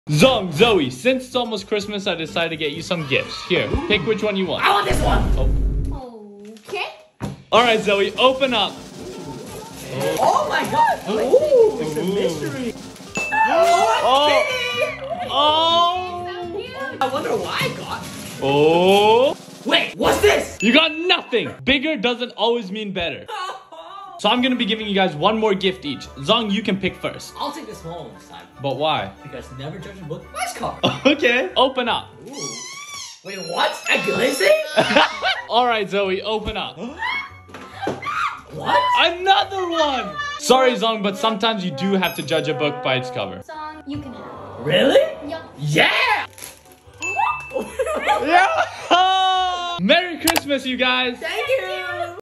Is BIGGER... Better? Zoe vs Zhong (Christmas)
Christmas video of Zhong and Zoe choosing between small and big gifts...